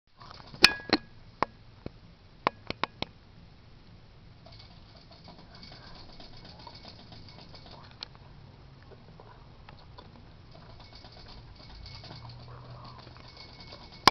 hamster banging the bell
some how she was ringing the bell for no reson ha